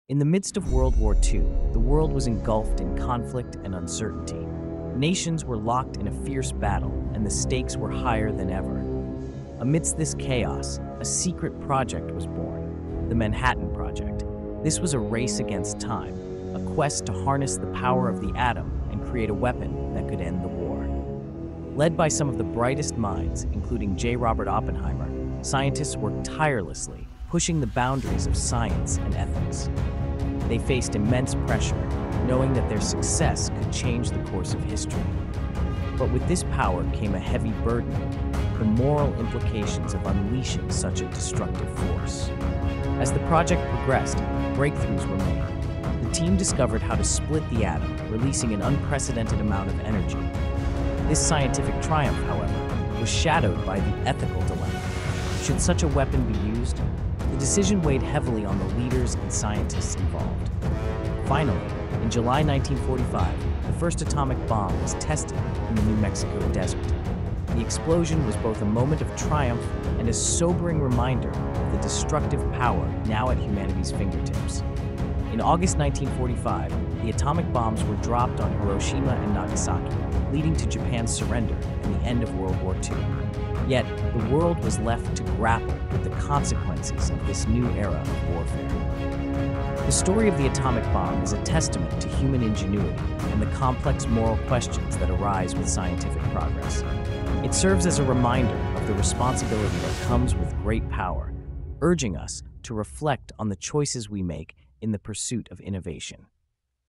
The Atomic Bomb: Why and When Was It Created? | Listen to History
The atomic bomb stands as one of the most consequential inventions in human history, both for its devastating power and its profound impact on global affairs. Developed during World War II under the United States' Manhattan Project, the bomb was first used in 1945 against the Japanese cities of Hiroshima and Nagasaki. These bombings resulted in immense destruction and loss of life, leading to Japan's surrender and the end of the war.